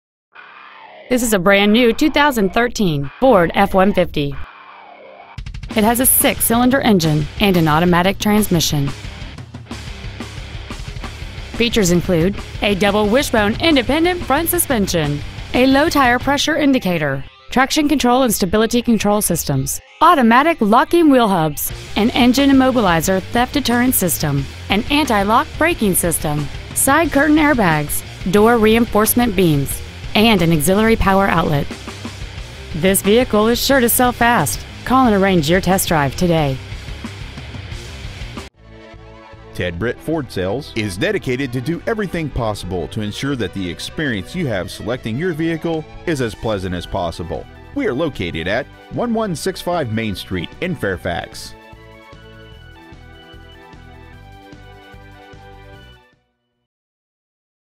2013 Ford F-150 Fairfax VA
Year : 2013
 Make : Ford
 Model : F-150
 Engine : 3.5L V6 24V GDI DOHC TWIN TURBO
 Trans . : 6-SPEED AUTOMATIC
 Exterior : STERLING GRAY

 2013 Ford F-150 Fairfax  VA